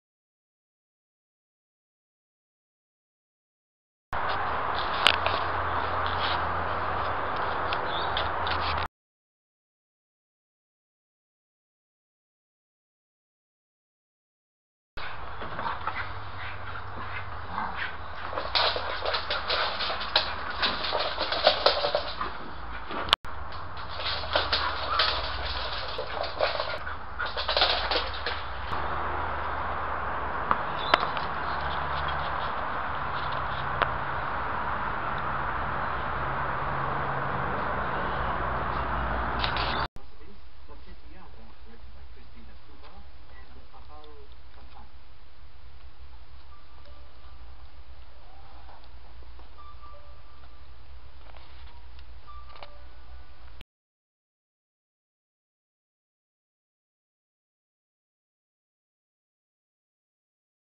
Frasier, a foster dog with Animal Welfare spent 10 days with Winston and I.
Frasier, a 2 year old foster dog from Animal Welfare League(AWL) spent 10 days with Winston and I.  He is a beautiful English Springer Spaniel, who had had a broken leg, and a plate in it which hadn't knitted, before being found as a stray.  While he was with me, I was medicating him for an infection in the wound.  I'm taking him back to AWL tomorrow where they'll operate on his leg.